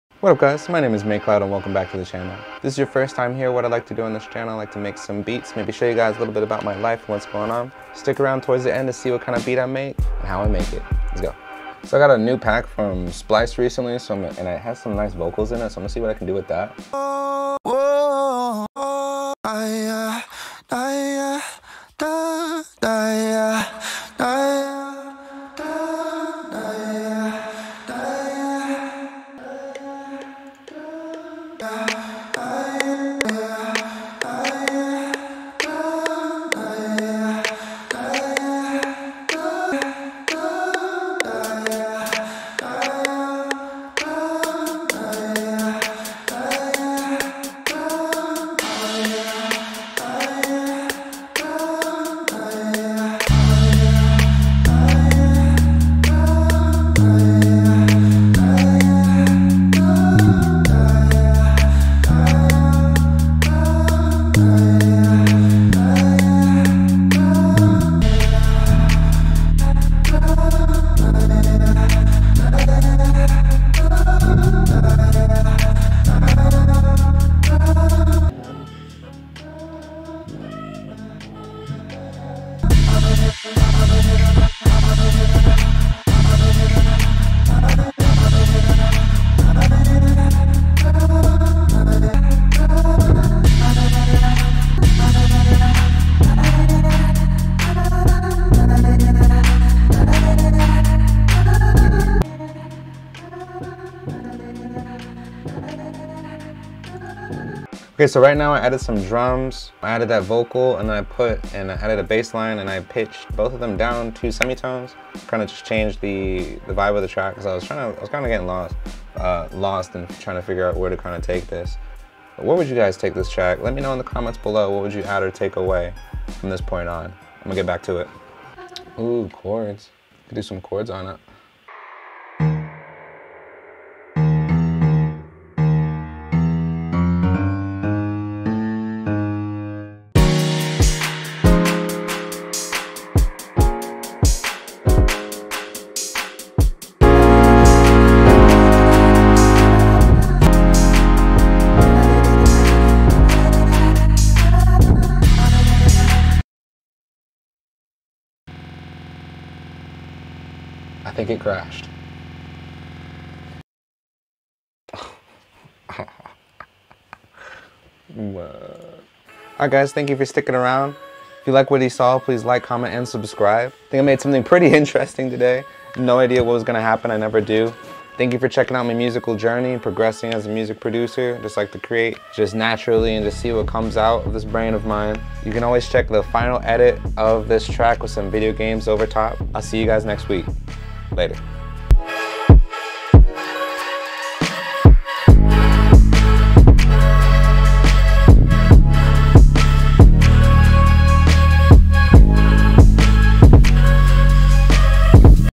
BEST VOCAL SAMPLE TRAP BEAT
Hey everyone!
I recently bought a new sample pack from splice and found some amazing vocal samples to sample in FL Studio 20! Check out what I make with it!